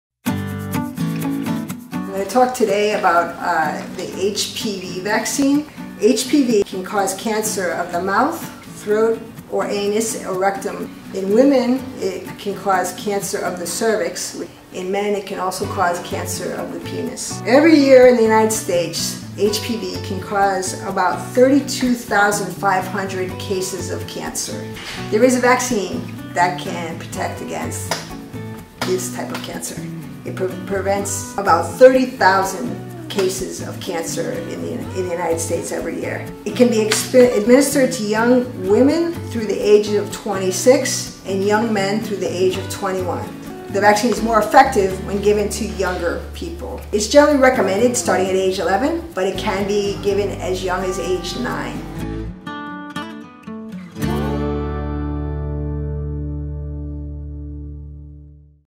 Human Papilloma Virus 1 minute (in English)
Every year in the United States, the Human Papilloma Virus or HPV causes around 32,500 cancers in women and men. The HPV vaccine can prevent the development of most cancers (around 30,000). It can prevent about 70% cervical cancer, 80% cancer, 60% cancer of the vagina and 40% cancer of the vulva.